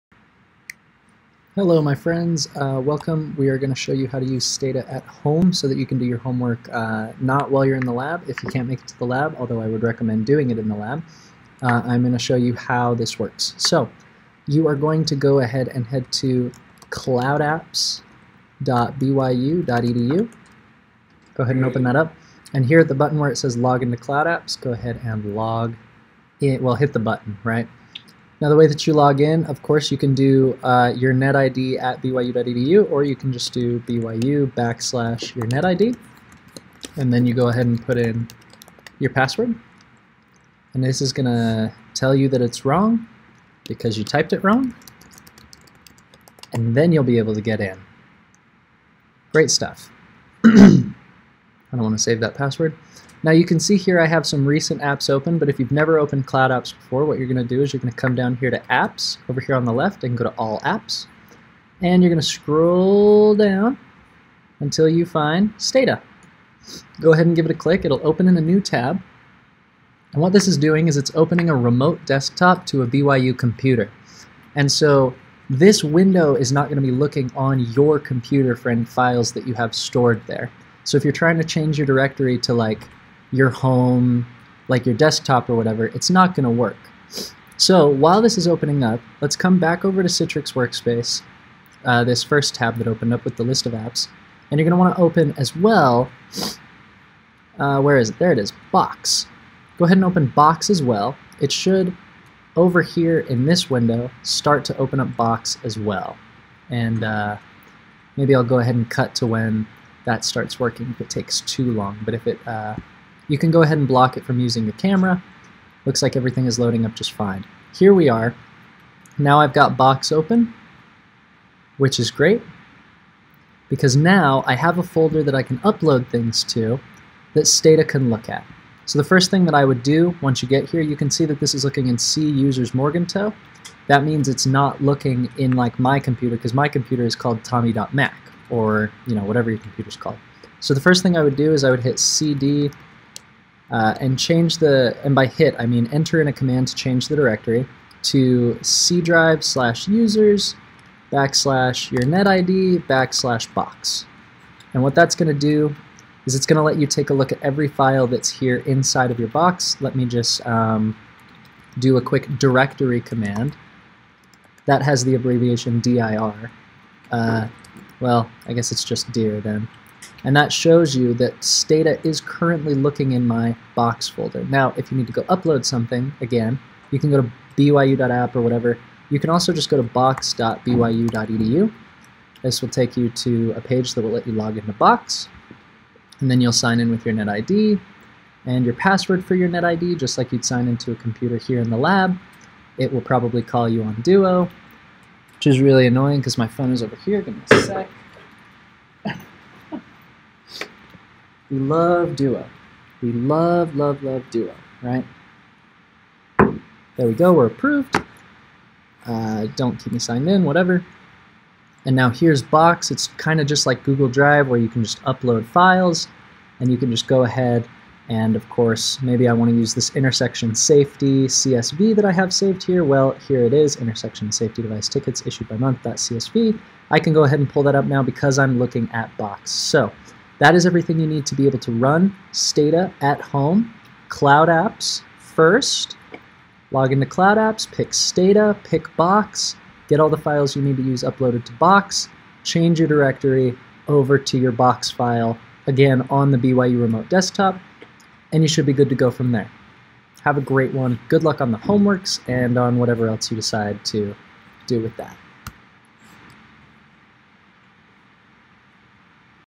How to Run Stata at Home in Under 5 Minutes - BYU CloudApps Tutorial
If you're a BYU student and want to use Stata (or many other softwares) at home instead of coming onto campus, this guide will get you set up in under 5 minutes!